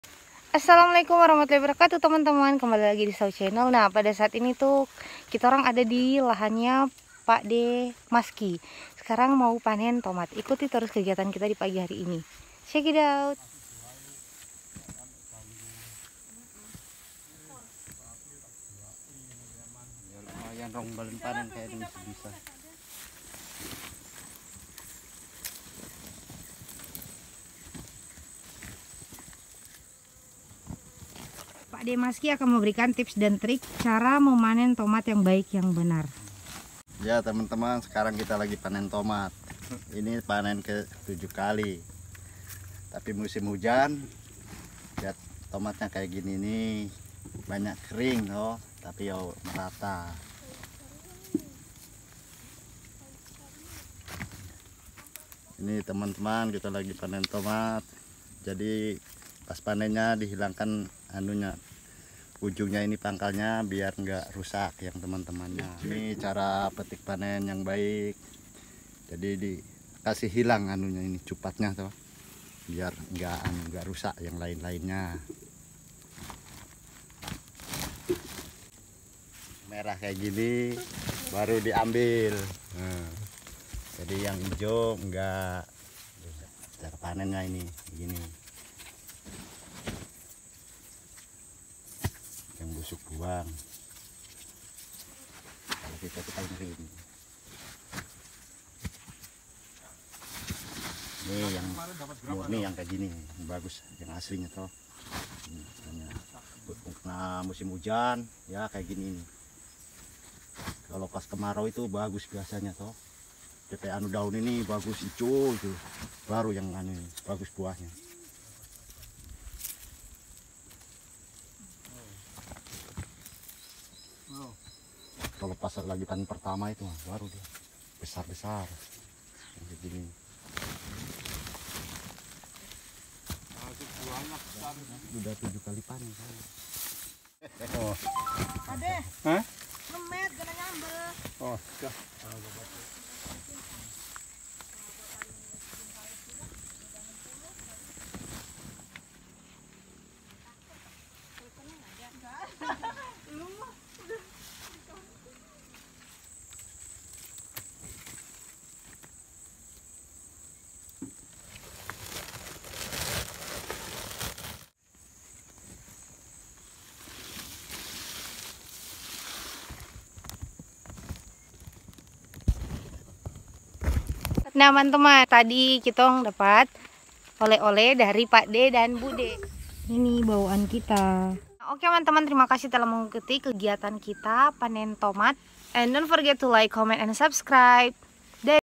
DATANG KOSONG PULANG BUNGKUS || N45iB B4iK ANAK S0L3H4
Semoga Terhibur 😁
Hallo semuanya, saat ini saya lagi berada di Lahan Tomat Bapak Maski bersama rekan-rekan Praktik Kerja Lapangan (PKL) II yang merupakan salah 1 program dari Kampus Politeknik Pembangunan Pertanian (Polbangtan) Manokwari, nah videonya yakni seru seruan kami di ladang Pakde Maski sambil panen tomat.
Yuk diikuti terus... Ditonton sampai habis ya!

Mohon maaf jika banyak kekurangan :)
**Hayati sarankan untuk menggunakan Earphone/Headset ya agar lebih jelas suaranya.
**Tetap Mematuhi Protokol Kesehatan Ya
**Terimakasih untuk semua Pihak yang begitu Baik